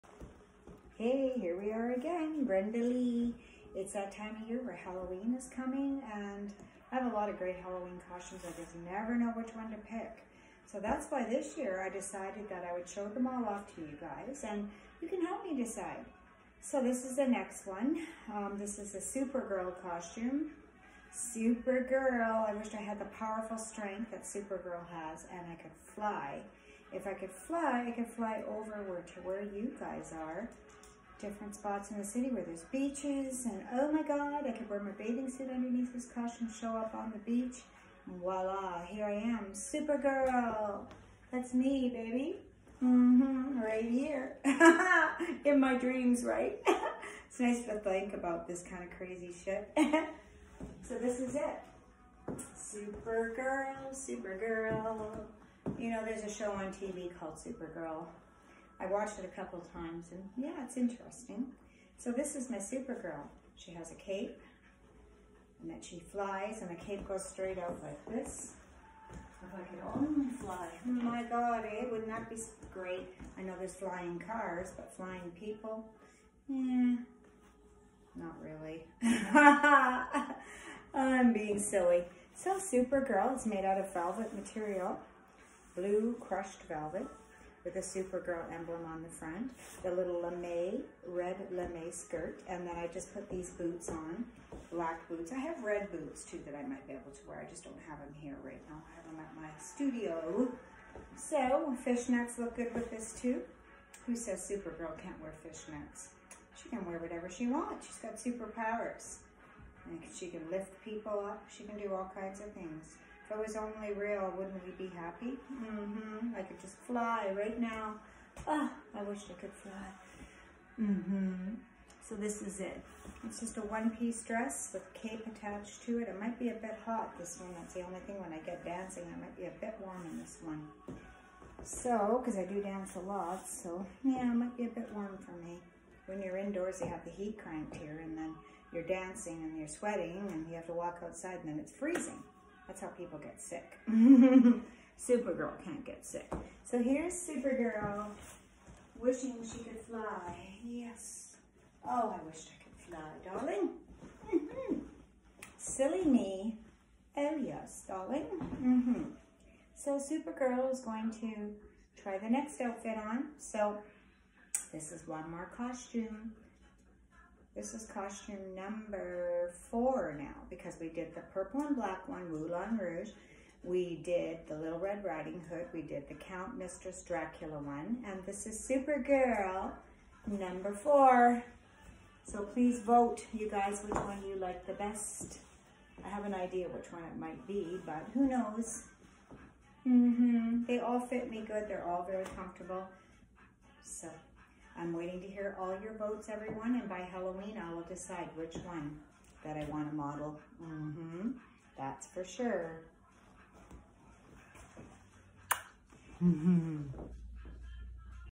Supergirl
This is a fun costume super hero super girl
If I could only fly￼
Blue crushed velvet with a gorgeous red foil skirt￼
Black fishnets￼
And you are ready to fly you’re ready to be a superhero￼
Enjoy Halloween please don’t forget to vote on which costume you like the best this is costume number 4 ￼